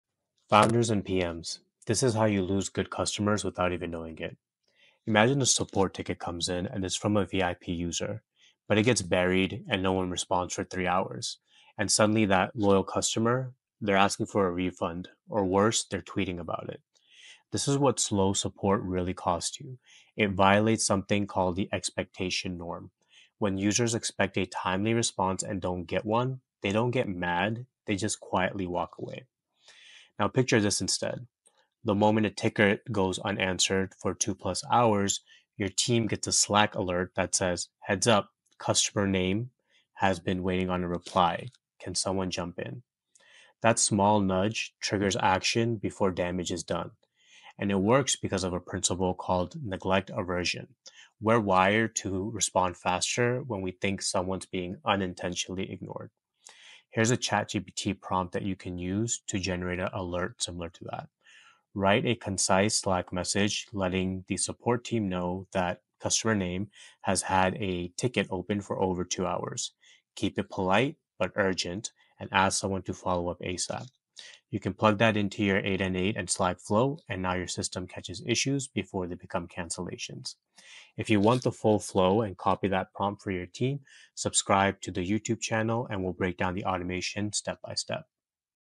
Use N8N + ChatGPT to shorten support delays
Support delays don’t just hurt. They compound.
One unanswered ticket from a VIP = lost trust, churn, or public complaints.

In this episode, we show how to:
⚡ Auto-alert your team on Slack when a ticket sits too long
⚡ Avoid missed follow-ups without checking dashboards
⚡ Build this into your agent-powered ops stack using N8N

No more buried tickets. No more preventable churn.

Not sure if you're ready for AI agents?
Fill out our "AI Team Readiness Assessment" for a personalized report.

Check out our Youtube channel for more on AI agent workflows.